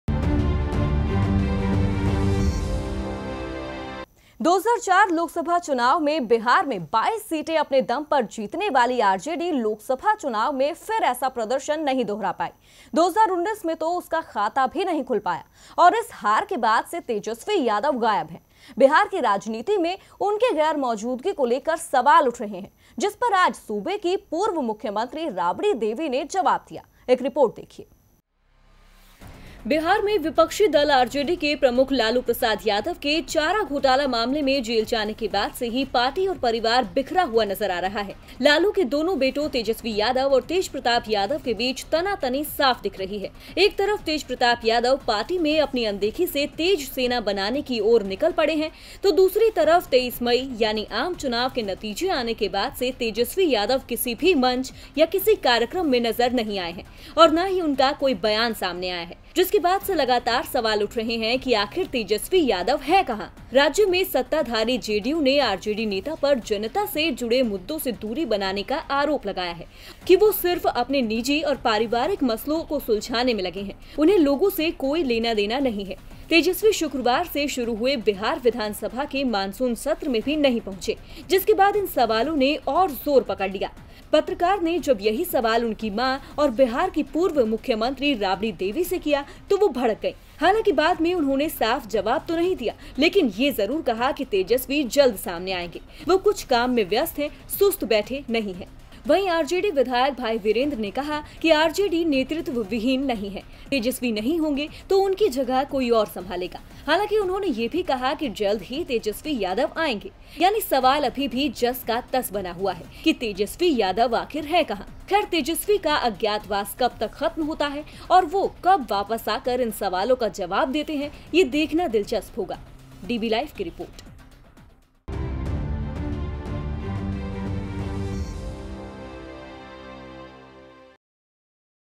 आखिर rabari devi को क्यों आया गुस्सा? Tejaswi yadav के गायब रहने पर बोलीं राबड़ी
आखिर rabri devi को क्यों आया गुस्सा? Tejaswi yadav के गायब रहने पर बोलीं राबड़ी